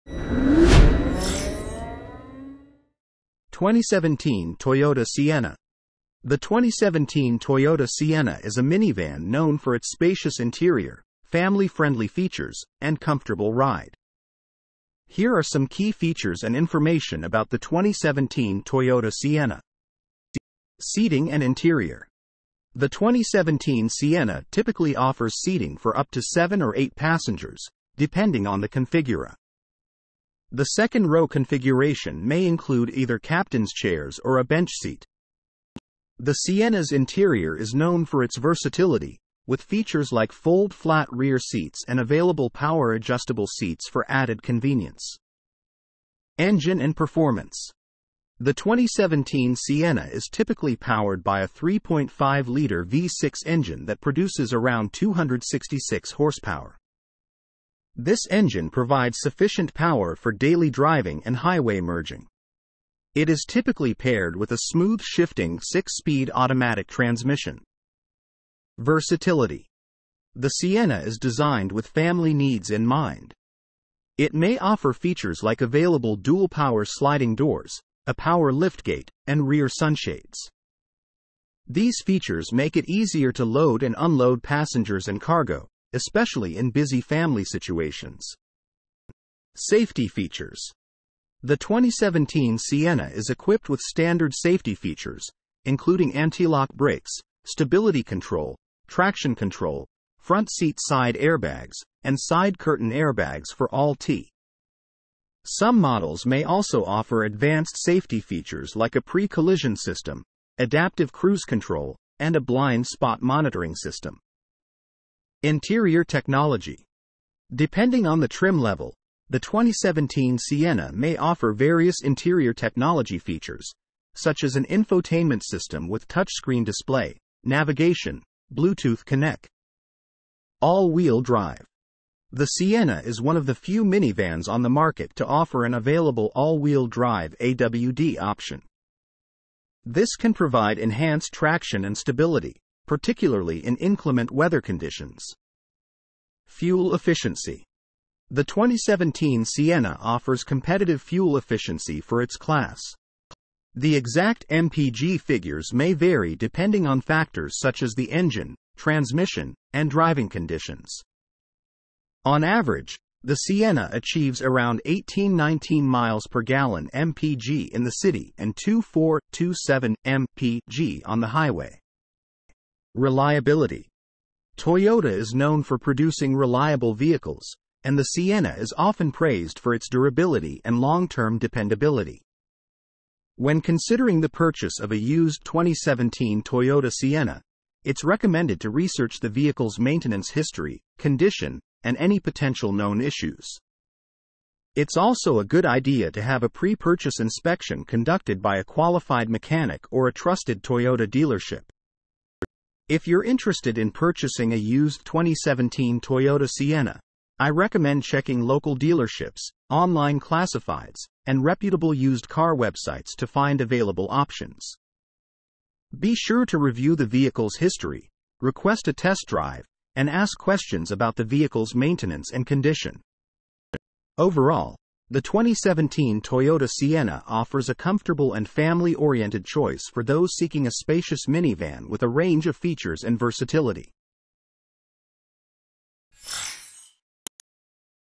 2017 Toyota Sienna
The 2017 Toyota Sienna is a minivan known for its spacious interior, family-friendly features, and comfortable ride.